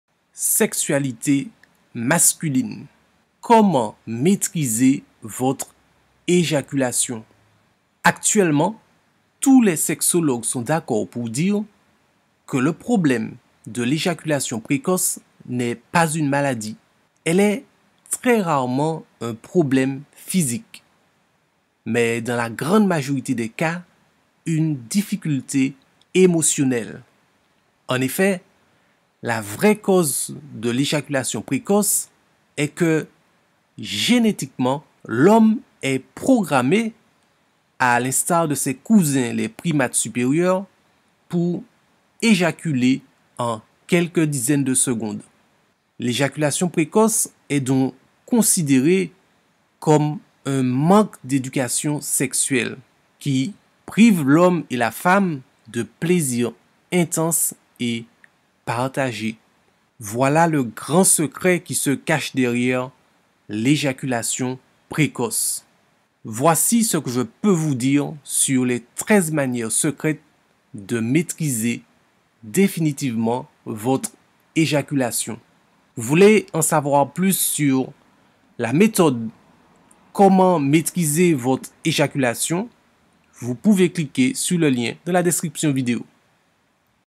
Comment maitriser votre éjaculation ?
Actuellement, tous les sexologues sont d’accord pour dire que le problème de l’éjaculation précoce n’est pas une maladie.

Elle est très rarement un problème physique.

Mais dans la grande majorité des cas une difficulté émotionnelle !

En effet, la vraie cause de l’éjaculation précoce est que génétiquement,

l’homme est programmé, à l’instar de ses cousins les primates supérieurs, pour éjaculer en quelques dizaines de secondes !

L’éjaculation précoce est donc considérée comme un manque d’éducation sexuelle qui prive l’homme et la femme de plaisirs intenses et partagés.

Voilà, le grand « secret » qui se cache derrière l’éjaculation précoce !

Voici ce que je peux vous dire sur les 13 manières “secrètes” de maîtriser définitivement votre éjaculation :

Avec le « secret » n°1 vous savez précisément QUOI faire quand vous êtes sur le point d’éjaculer. La plupart des hommes (même sans problème) ne savent pas contrôler ce moment fatidique. C’est leur corps qui est le MAITRE du jeu ! Mais avec ce premier « secret » que j’ai découvert dans une revue scientifique réputée, c’est VOUS qui déciderez quoi faire ! Ejaculer ou prolonger le plaisir de faire l’amour !

Le « secret » n°2 vous dévoile les 3 muscles de votre corps qui vous OBLIGENT à éjaculer si vous ne savez pas les détendre. Dans cette partie, j’ai découvert comment maîtriser la décontraction de ces muscles instantanément (et sans que ma partenaire ne sache ce que je fais). Je vous montre 2 vidéos qui vous expliquent, pas à pas, comment vous pouvez également y arriver et être certain d’éjaculer quand VOUS l’avez décidé. Gardez déjà à l’esprit que la capacité à se détendre est fondamentale pour atteindre votre objectif !

Le « secret » n°3 vous montre une façon très précise (et facile) de respirer pour retarder l’éjaculation le plus longtemps possible. Tant que vous respirez comme cela, vous avez le contrôle ! Cette méthode a été créée par le professeur François de Carufel, le premier sexologue à avoir mis au point une méthode innovante contre l’éjaculation précoce… et à l’avoir expérimentée sur des MILLIERS d’hommes.